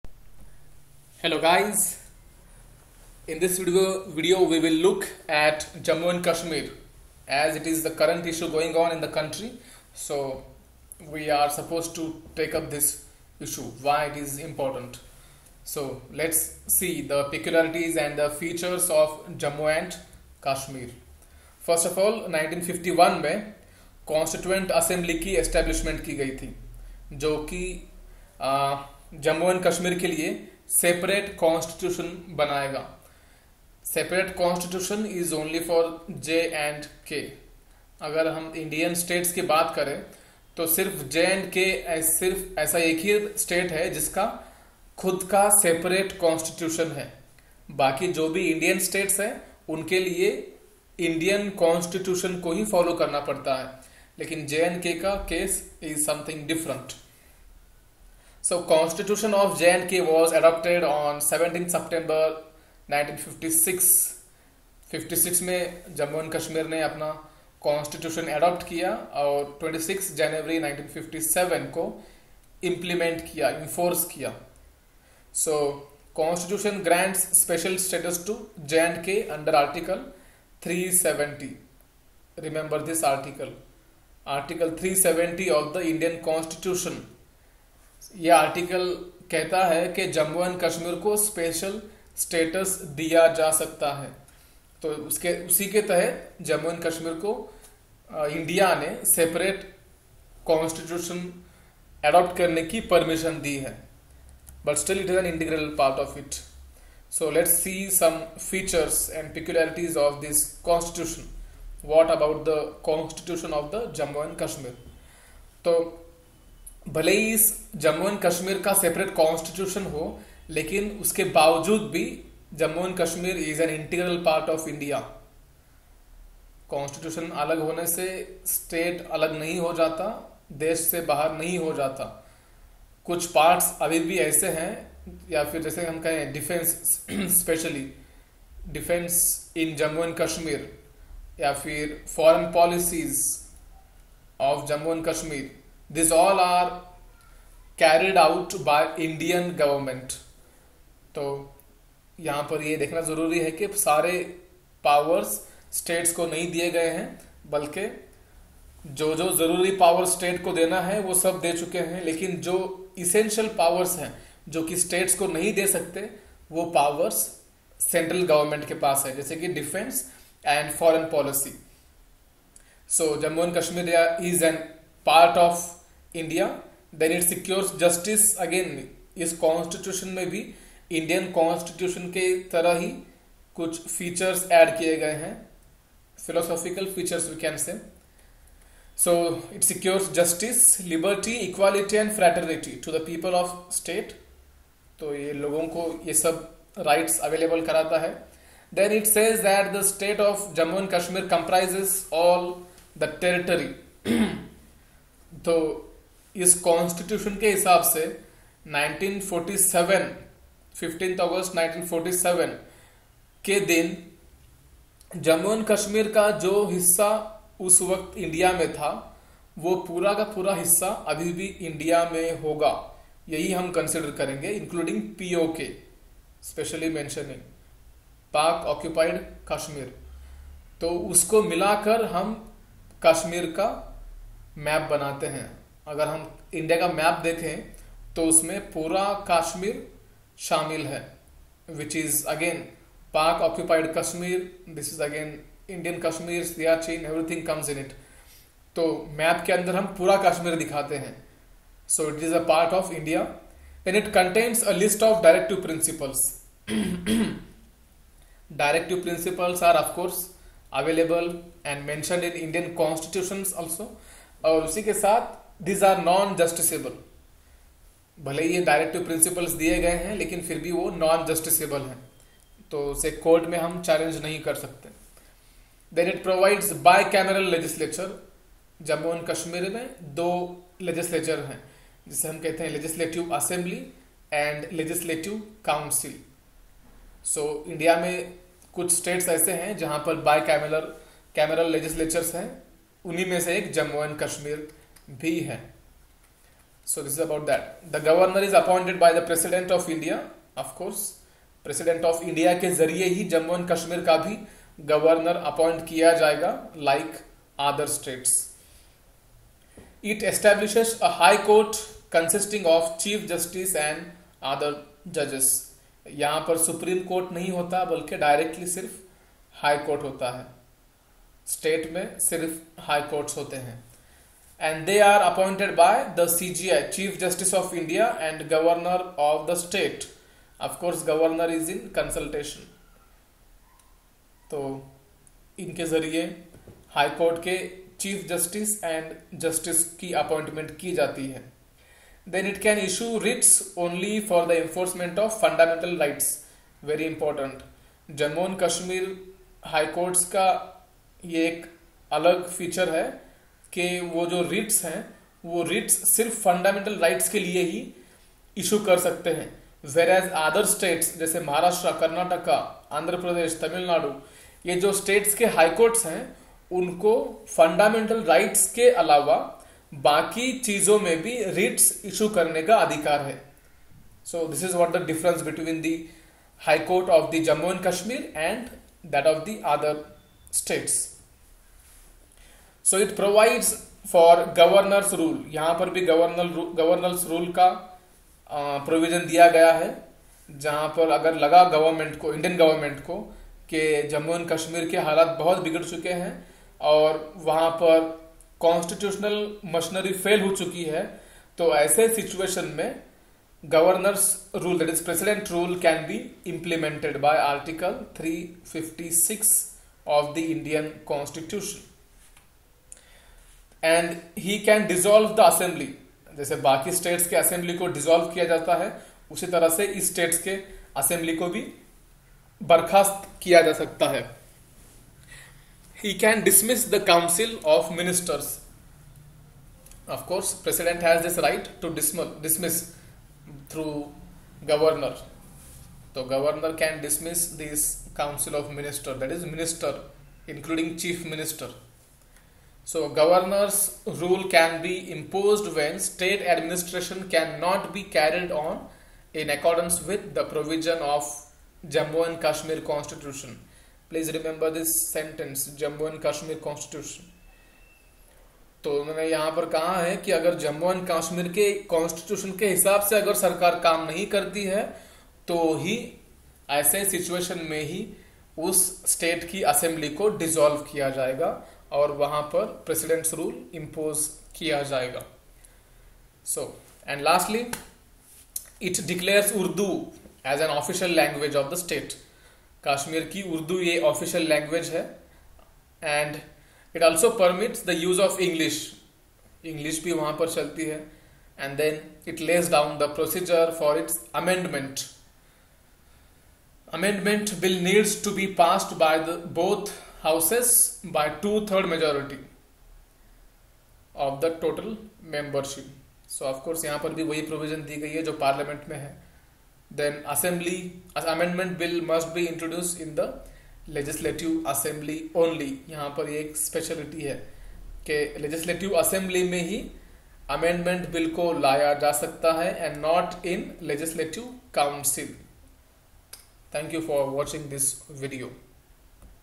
Kashmir - Special Provisions?
This video explains in detail about features of Jammu & Kashmir
For unlimited Mock Tests and Video Lessons, login to our official